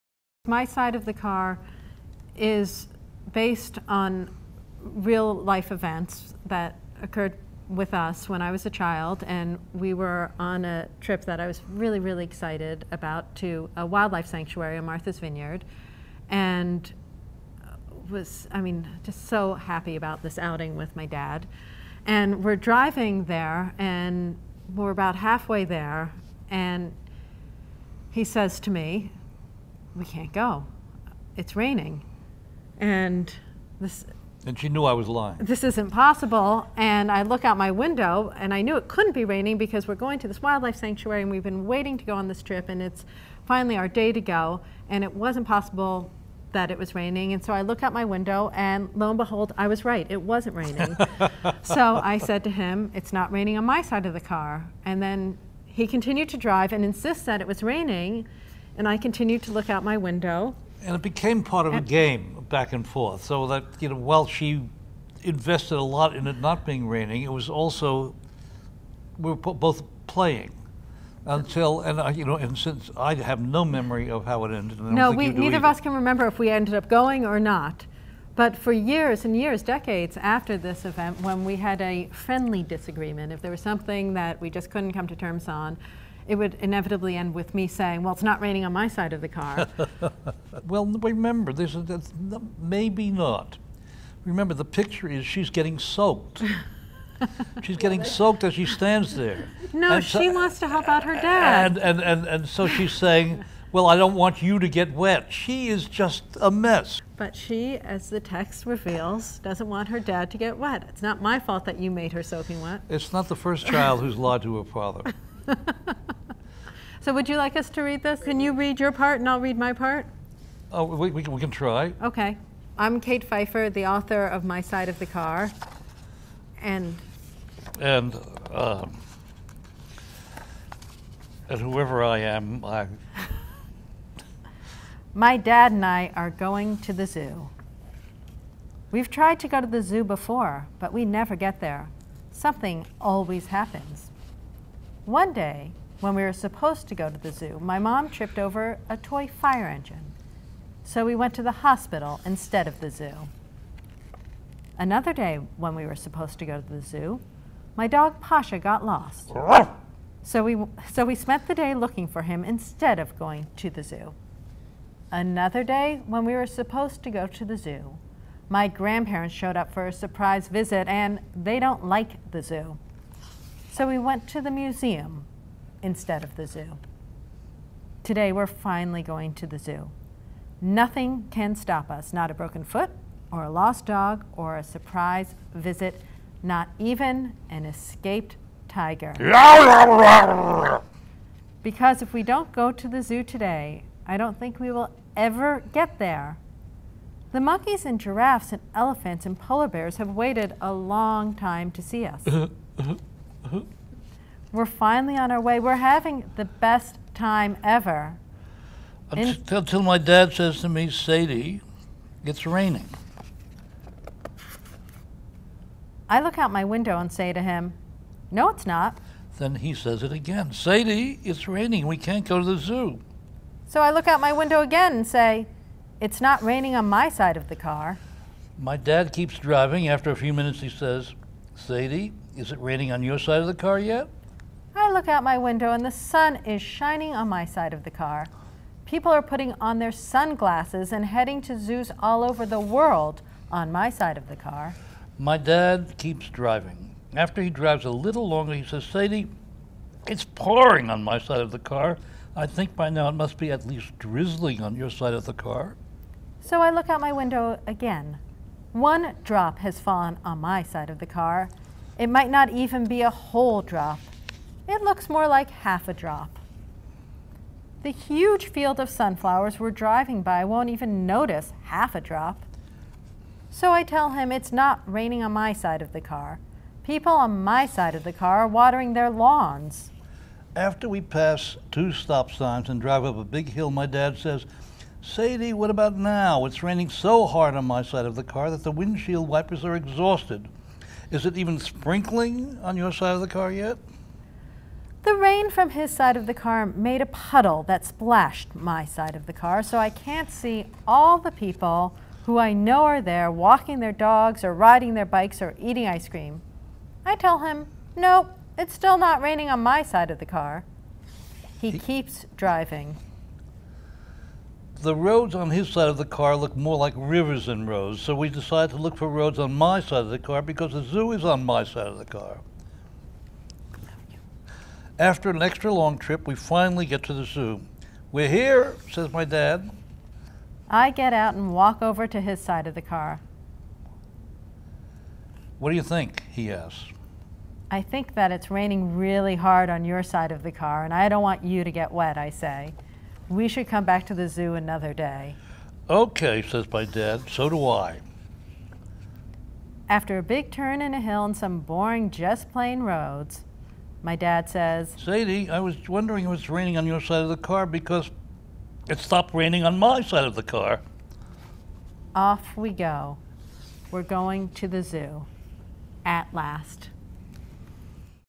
My Side of the Car
Kate and Jules Feiffer read their book My Side of the Car together. This story, too, was based on a real life event -- an outing with dad that kept getting interrupted.